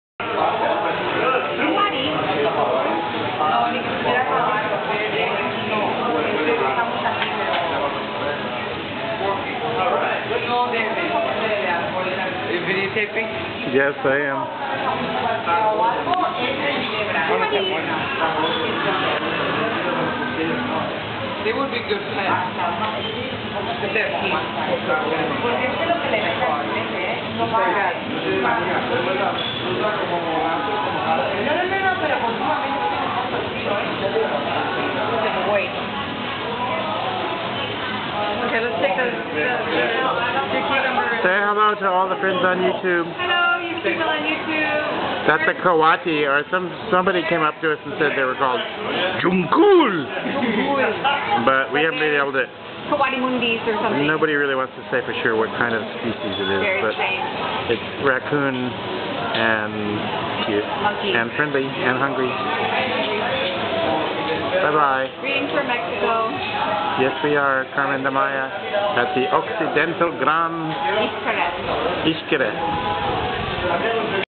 Julian and the coati, also known as a tejón (not junkul)
At the Occidental Grand Xcaret hotel in Quintana Roo, northeastern tip of the Yucatan peninsula, Mexico.

filename: 20110930-Mexico-116.MOV